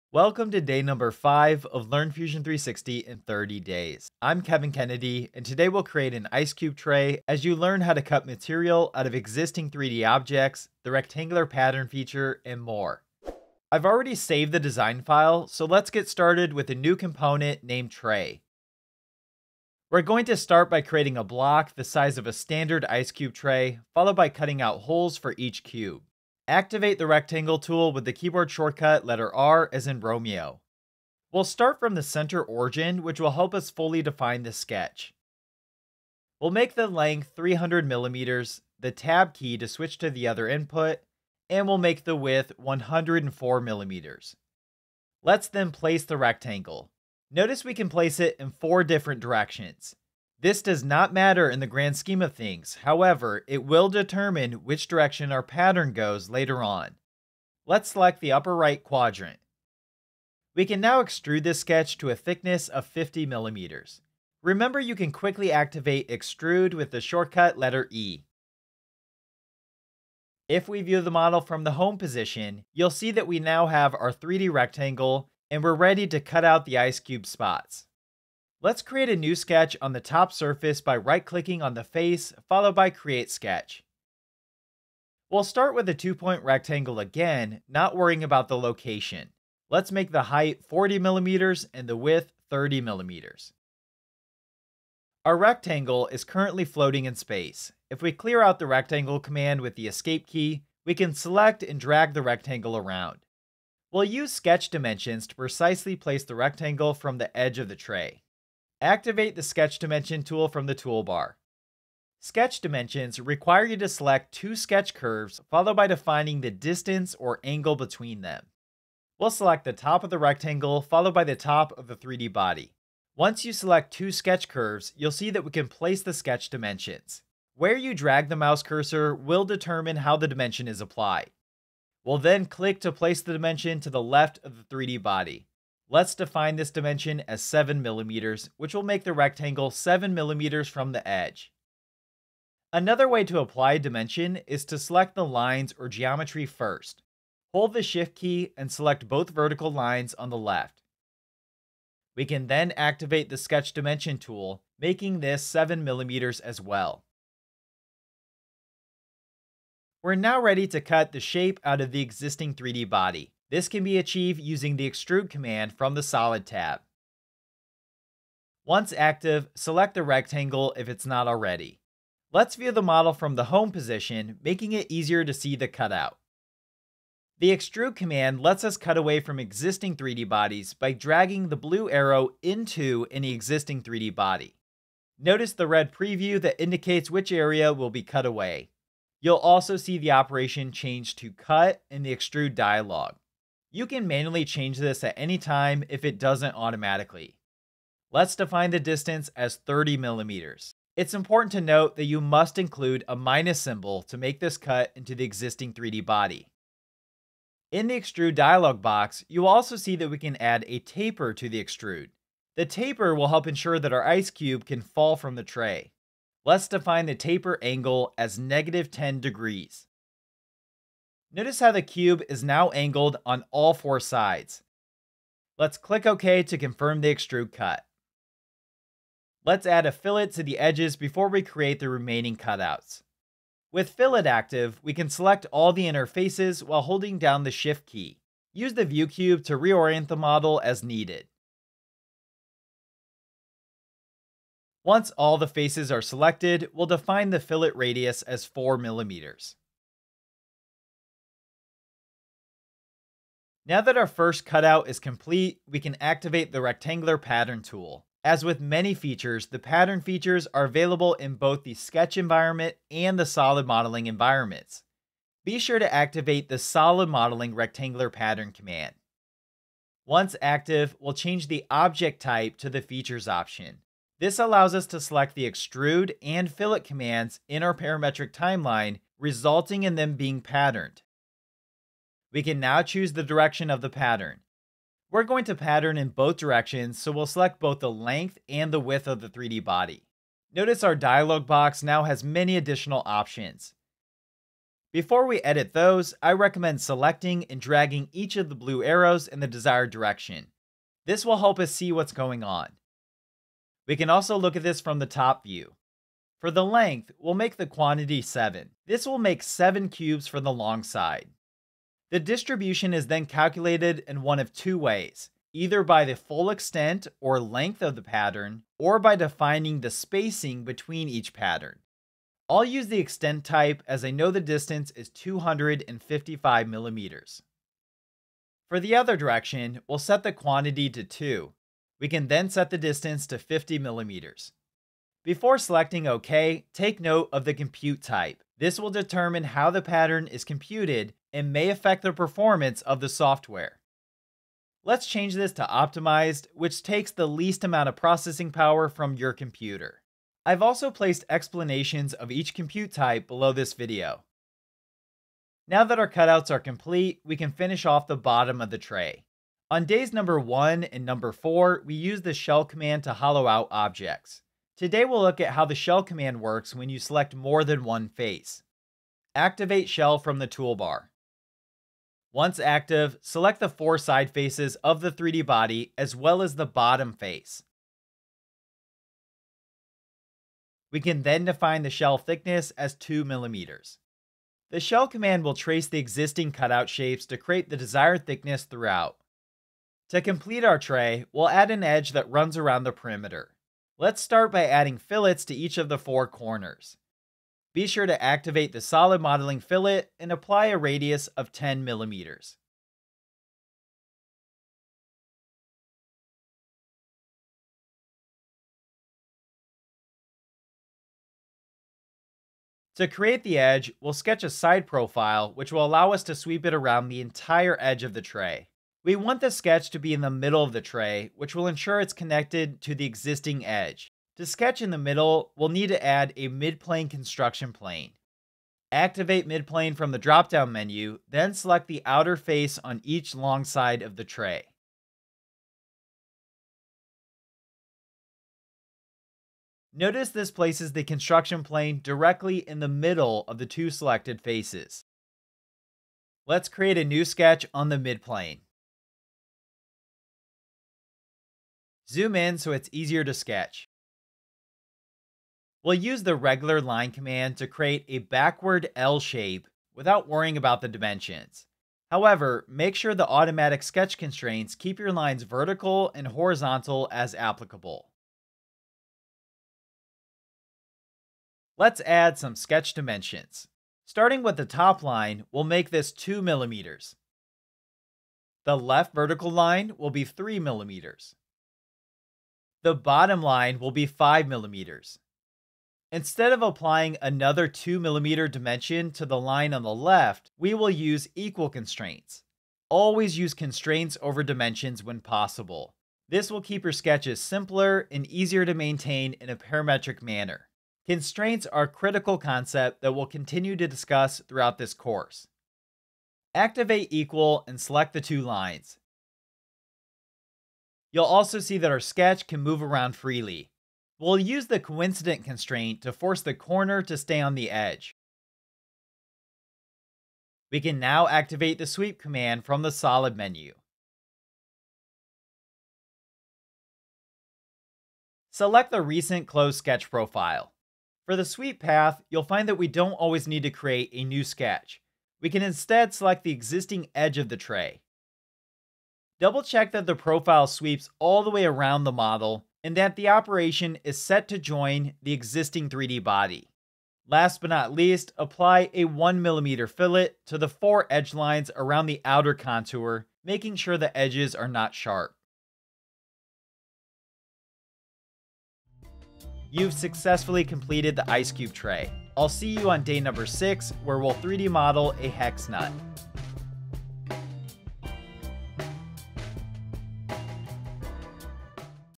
Day 5 of Learn Fusion 360 in 30 Days for Complete Beginners! - 2023 EDITION
New to Fusion 360? This is a brand new 2023 version of Day #5 of "Learn Fusion 360 in 30 Days for Complete Beginners" - the most popular Fusion 360 course on the internet!

TIMESTAMPS
00:00 - Day 5 Model an Ice Cube Tray in Autodesk Fusion 360
00:36 - Sketch the overall shape with a rectangle
01:21 - Create New Sketch on Planar Surface in Fusion 360
02:38 - How to Extrude Cut in Autodesk Fusion 360
03:29 - Add Taper Angle to Extrude in Fusion 360
04:19 - How to use Rectangular Pattern in Fusion 360
05:54 - How to Shell with more than one Face in Fusion 360
07:10 - Create a Midplane Construction Plane in Fusion 360
07:37 - Sketch the Lip of the Ice Cube Tray in Fusion 360
08:46 - Sweep the Lip of the Ice Cube Tray in Fusion 360
09:23 - Next Lesson in this course